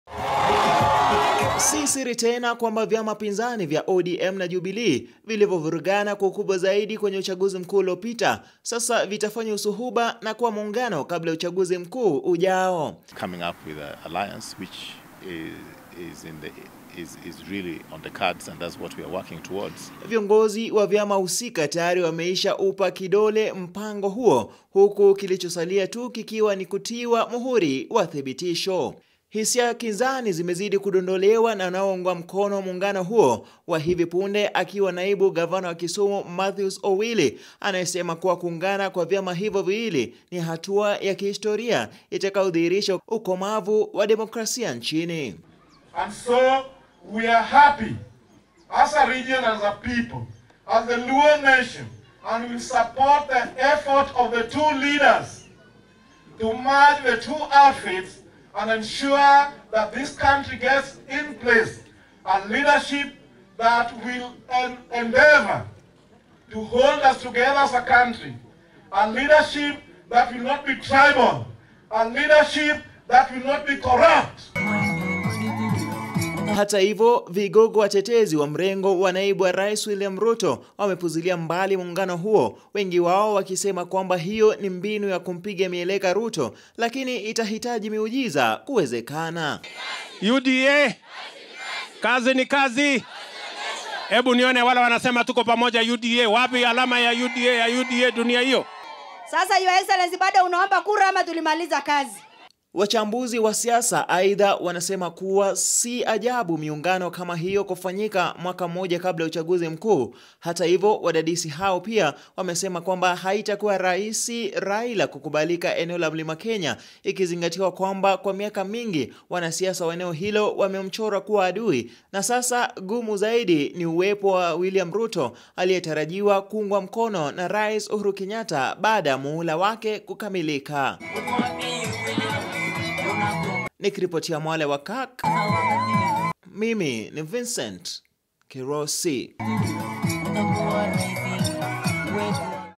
USUHUBA WA ODM NA JUBILEE
HISIA TOFAUTI ZINAENDELEA KUTOLEWA NA VIONGOZI MBALIMBALI KUHUSU MPANGO WA CHAMA TAWALA CHA JUBILEE KUFANYA MUUNGANO NA KILE CHA ODM KABLA YA UCHAGUZI MKUU UJAO. VIONGOZI WANAOUNGA MKONO RAIS UHURU KENYATTA NA KINARA WA ODM RAILA ODINGA WAMECHANGAMKIA UWEZEKANO HUO HUKU WALE WA MRENGO WA NAIBU RAIS William Ruto WAKISEMA MPANGO HUO NI SAWA TU NA TONE KWENYE BAHARI KUU LA NAIBU RAIS WILLIAM RUTO...